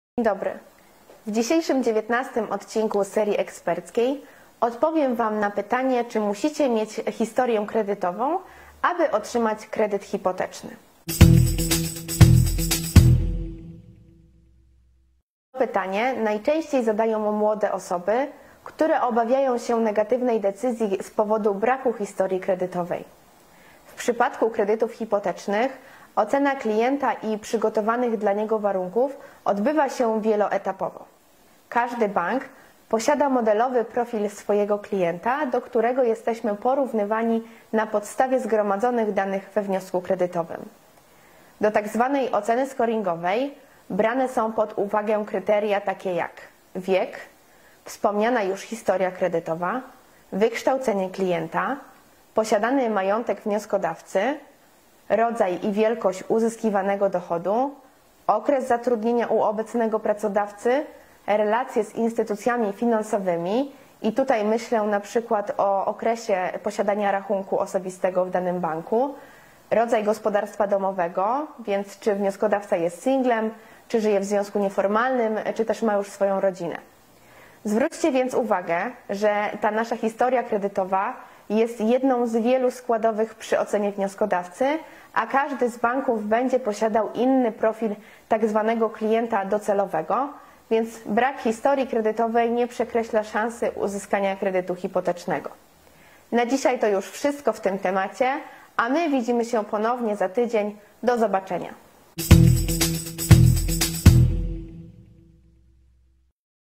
Historia kredytowa - Kredyty hipoteczne - odcinek 19
Historia kredytowa - czy jest konieczna do otrzymania kredytu hipotecznego  W 19. odcinku naszej serii eksperckiej o kredytach hipotecznych Agnieszka Maniewska wyjaśnia:
- czym jest historia kredytowa,
- co to jest modelowy profil klienta
- jakie elementy brane są pod uwagę przez banki do oceny zdolności kredytowej.

Skontaktuj się z nami, jeśli rozważasz wzięcie kredytu hipotecznego. Pamiętaj, że NIE pobieramy prowizji od kredytów hipotecznych.